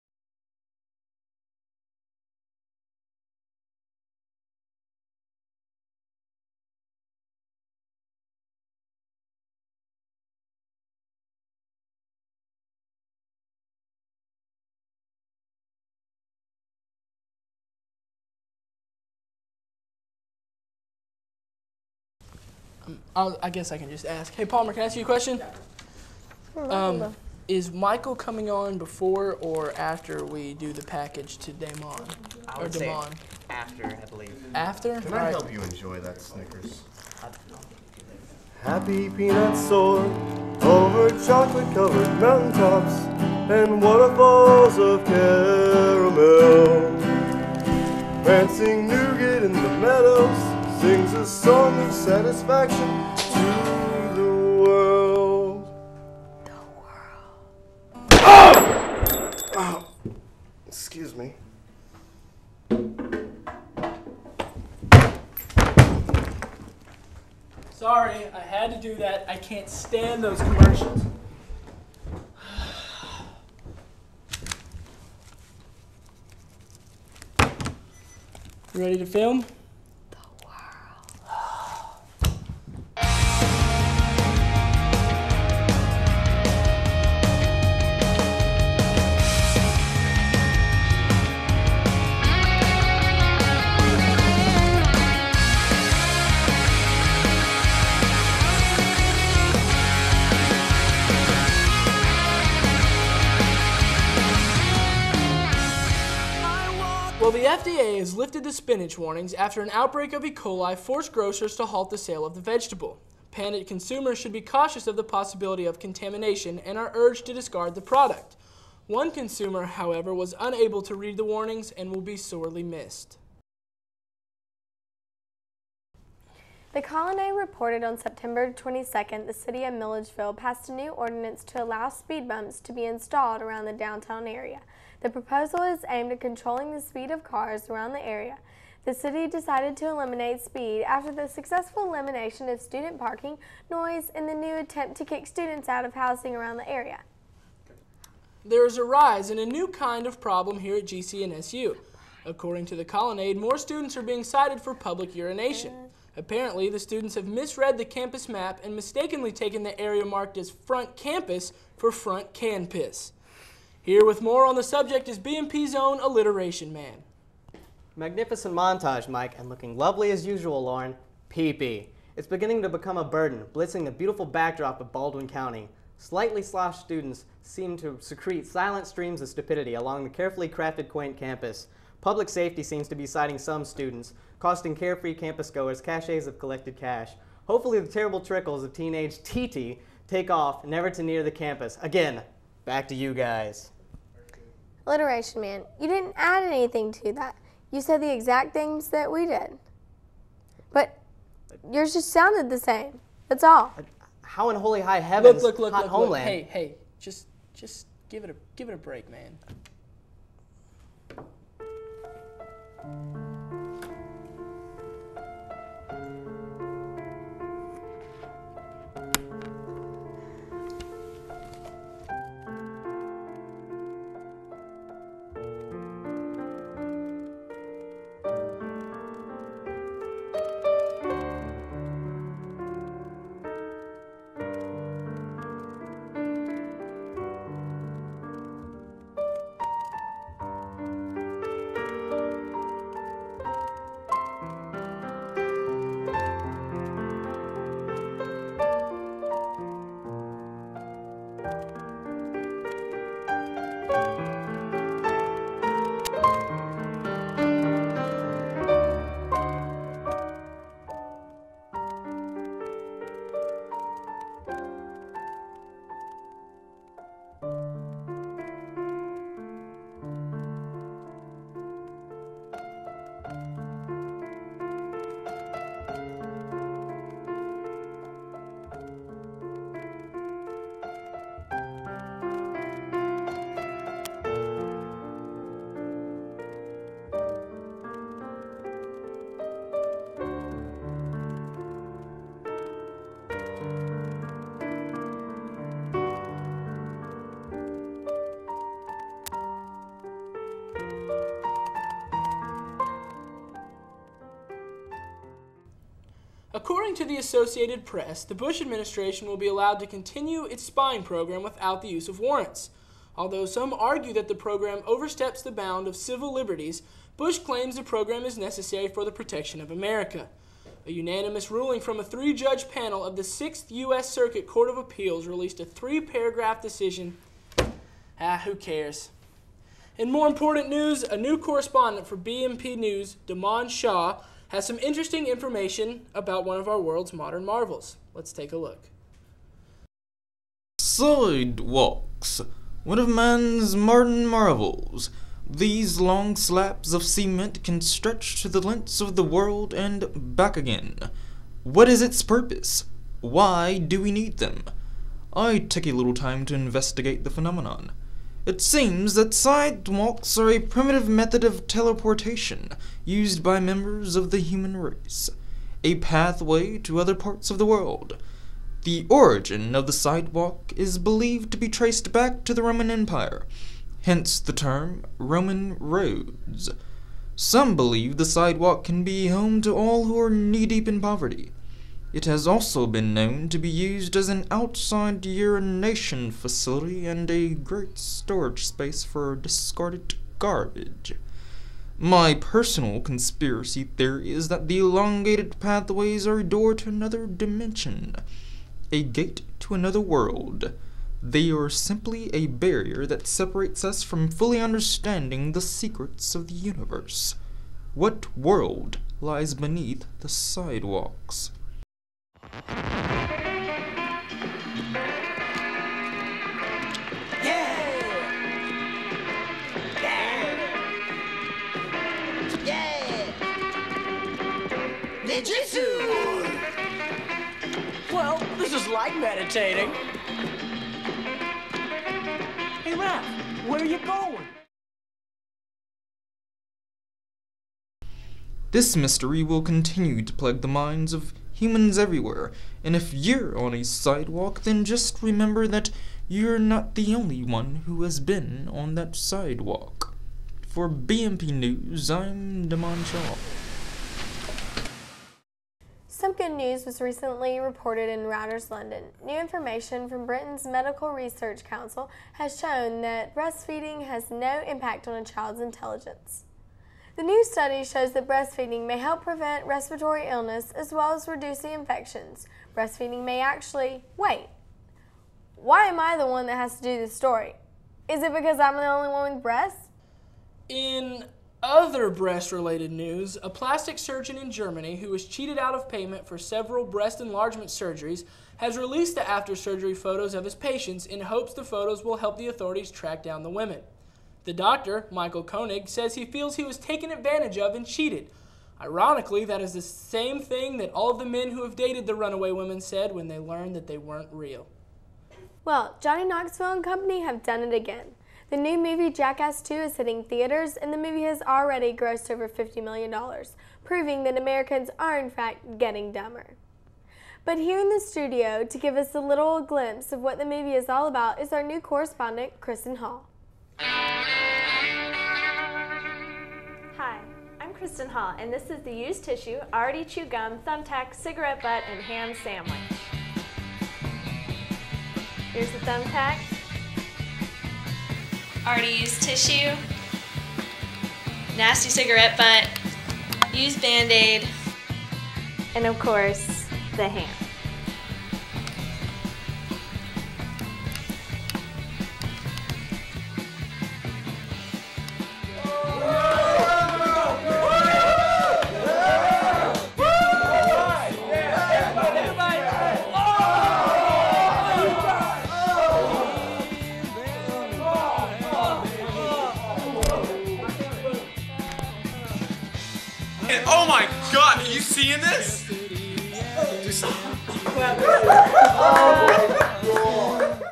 BMP News Episode # 2 - Part 1
Mock News show produced by students of Georgia College & State University